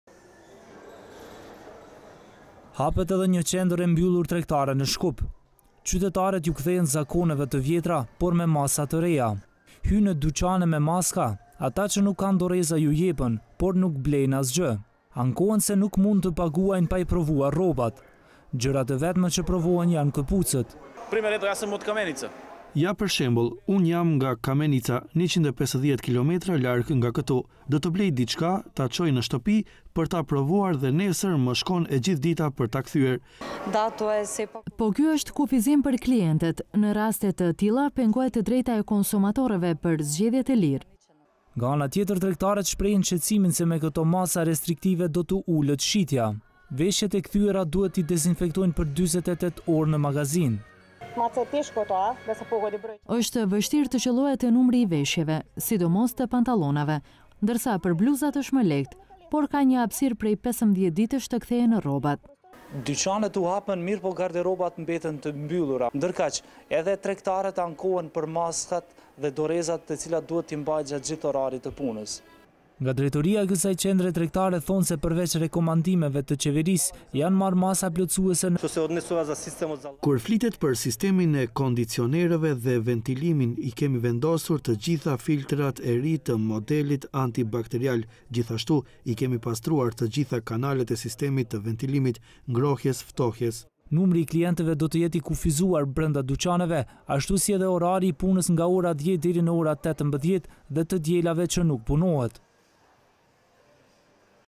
Qendrat e mbyllura tregtare rinisën punën
Hapet edhe një qendër e mbyllur tregtare në Shkup. Qytetarët iu kthehen zakoneve të vjetra, por me masa të reja. Hynë në dyqane me maska, ata që nuk kanë doreza iu jepen, por nuk blejnë asgjë. Ankohen se nuk mund të paguajnë pa i provuar rrobat. Gjërat e vetme që provohen janë këpucët.

“Ja për shembull unë jam nga Kamenica 150 km larg nga këtu. Do të blej diçka ta çoj në shtëpi për ta provuar dhe nesër më shkon e gjithë dita për ta kthyer. Një fanellë e thjeshtë do më kushtoj 50 euro!”.

“Po ky është kufizim për klientët në rastet të tilla pengohet e drejta e konsumatorëve për zgjedhje të lirë”.

Nga ana tjetër tregtarët shprehin shqetësim se me këto masa restriktive do t’u ulet shitja. Thonë se harxhojnë kohë shumë e shesin pak. Veshjet e kthyera duhet t’i dezinfektojnë për 48h në magazinë.

“Është vështirë të qëllohet numri i veshjeve, sidomos të pantallonave, ndërsa për bluzat është më lehtë. Por, ka një hapësirë prej 15 ditësh të kthehen rrobat”.

“Dyqanet u hapën, por gardërobat mbeten të mbyllura, ndërkaq edhe tregtarët ankohen për maskat dhe dorezat, të cilat duhet t’i mbajnë gjatë gjithë orarit të punës”.

Nga drejtoria e kësaj qendre tregtare thonë se përveç rekomandimeve të qeverisë, janë marrë masa plotësuese në mënyrë që të mos rrezikohet shëndeti i të punësuarve dhe klientëve.

Kur flitet për sistemin e kondicionerëve dhe ventilimit i kemi vendosur të gjitha filtrat e ri të modelit anit-bakteriale, gjithashtu i kemi pastruar të gjitha kanalet e sistemit të ventilimit, ngrohjes-ftohjes - tha Xheladin Xhuli, Drejtor i Qendrës Tregtare.

Sipas protokollit për funksionimin e qendrave të mbyllura tregtare, maskat, dezinfektuesit dhe distanca sociale duhet patjetër të respektohen. Numri i klientëve do të jetë i kufizuar brenda dyqaneve ashtu si edhe orari i punës nga ora 10-18 dhe të dielave nuk do të punojnë.

Adnan Qaili